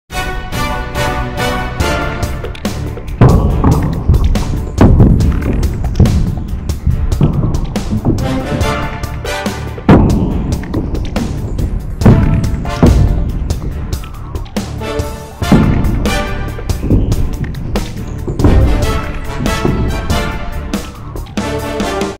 The Under Over Tube Shot - Slow Motion
Just a neat shot I evented 4 years ago in slow motion. It involves 4 balls, a paper towel tube and a toilet paper tube. I call it the Over Under Tube Shot. I shoot a ball Under the tubes and into the pocket. The two balls on top of the tubes fall down and hit each other and go into the pockets too, as the cue ball them jumps Over the last ball and into the opposite side pocket.